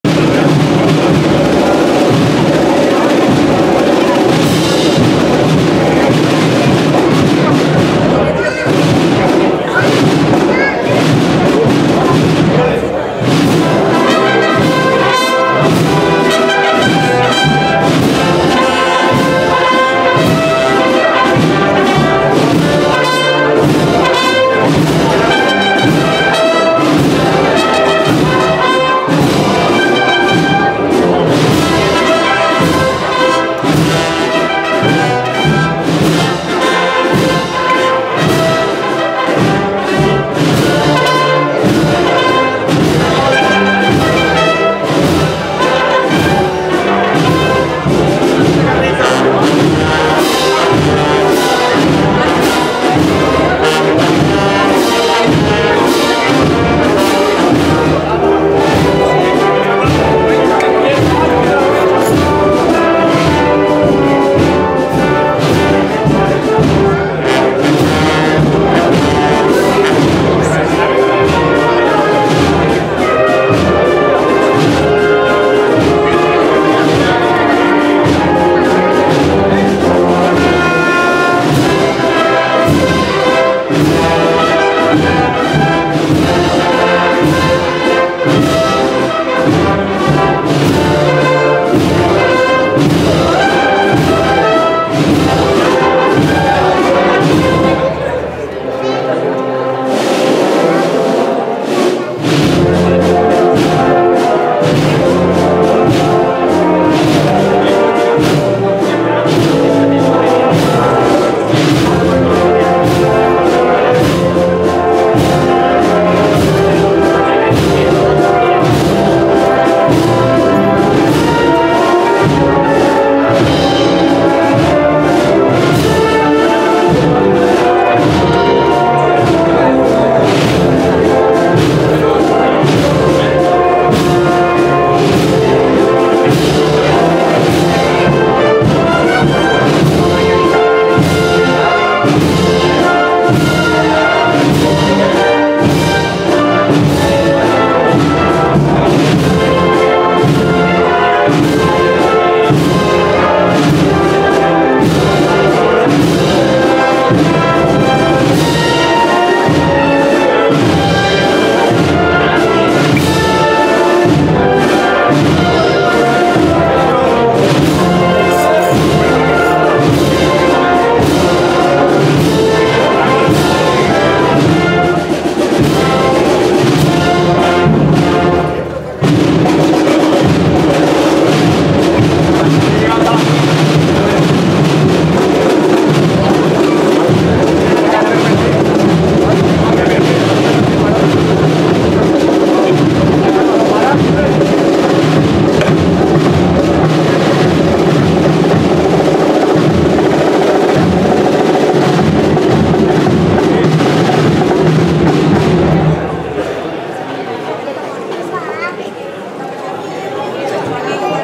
Salida de la Virgen del Rosario de los Humeros desde Santa Rosalía 2016
Salida de la Virgen del Rosario de los Humeros, desde elConvento de Santa Rosalía, en la mañana del 12 de Octubre del 2016